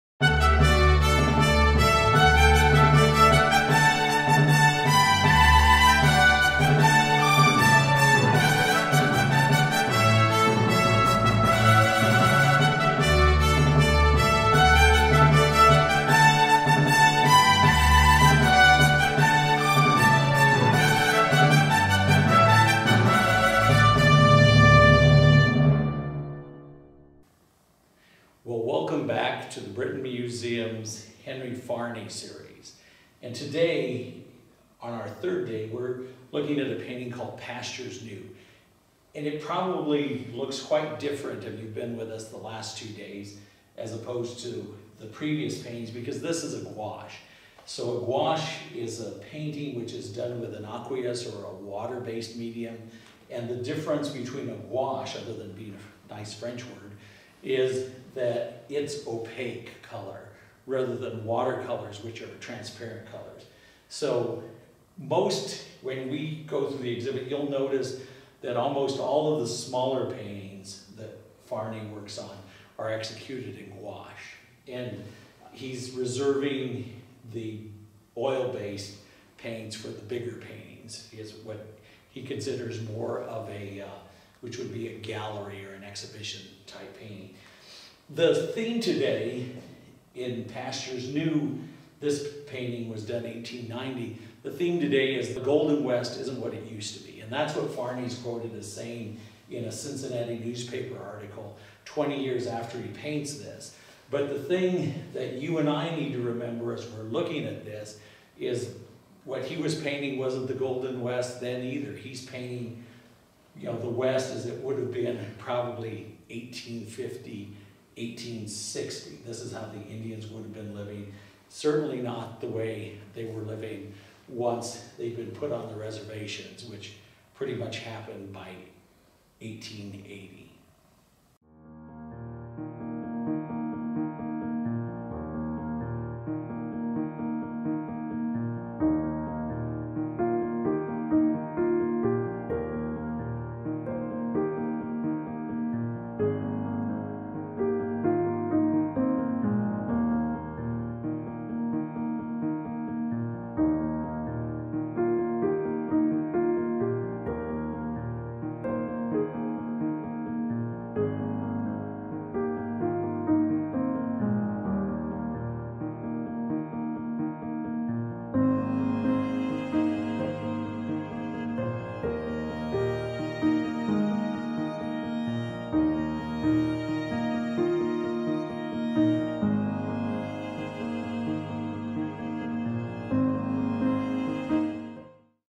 Henry Farny Episode 3 - Pastures New
The Brinton Museum presented one painting each week, beginning in May 2020 throughout the summer, from the George A. Rentschler Western Art Collection. The series features paintings by Henry Farny rom the American Heritage Center at the University of Wyoming. The George A. Rentschler Western Art Collection was on exhibit at The Brinton Museum through Labor day 2020.